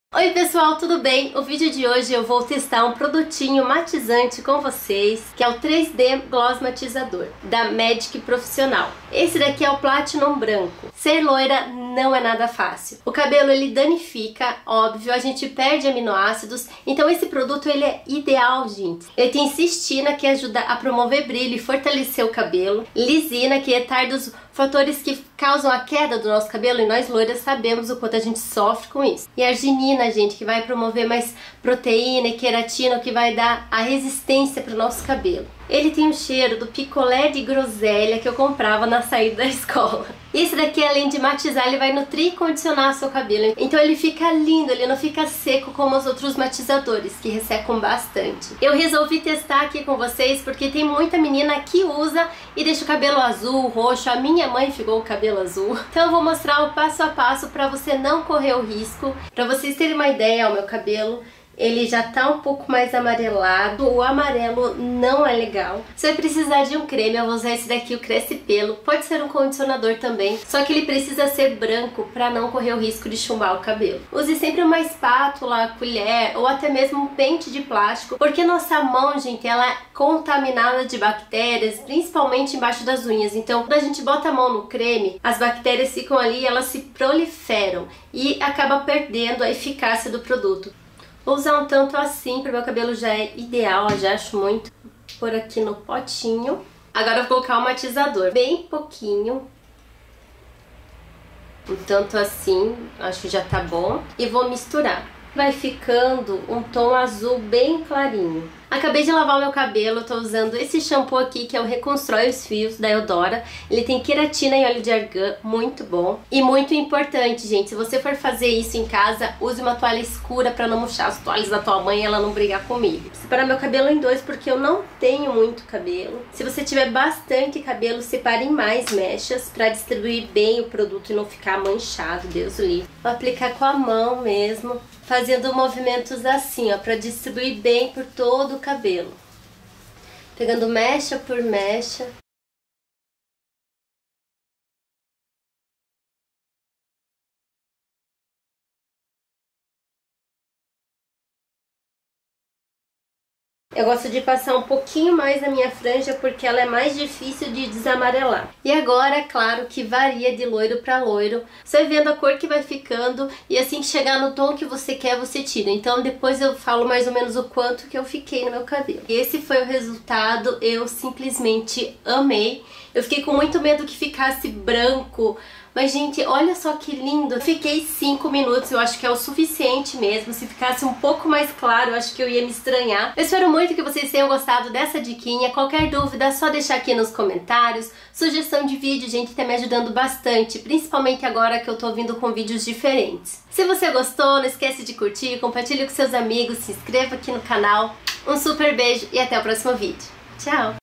Testando Gloss Matizador 3D
Resolvi gravar explicando o passo a passo de como matizar o cabelo e já aproveitei para testar o Gloss Matizador 3d da Magic Profissional,
pois muitas meninas aplicam errado e acabam chumbando o cabelo.
Espero que vocês gostem fiz com muito amor e carinho ♡

Qualquer dúvida ou sugestão de vídeo deixe aqui nos comentários.